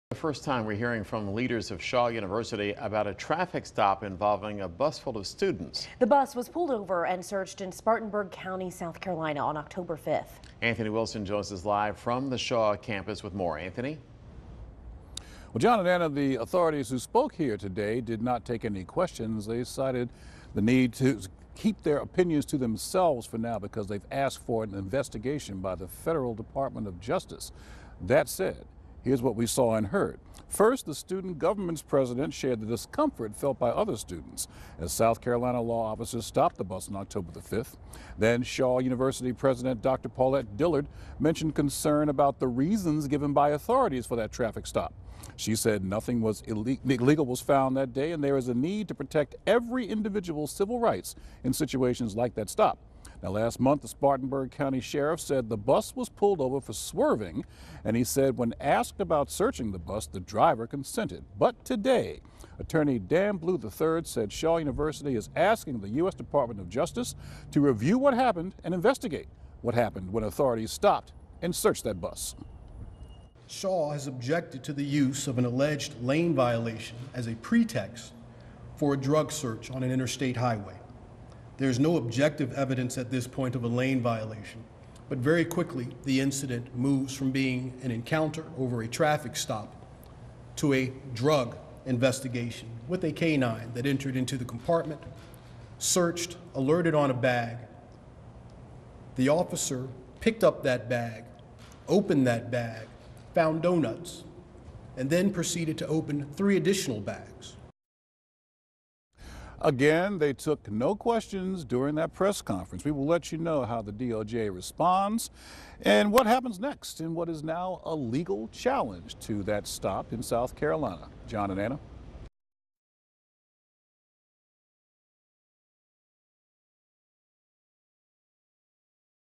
Shaw University leaders address traffic stop involving students
Shaw's president spoke at the press conference where it was revealed the university has filed a formal complaint with the Department of Justice.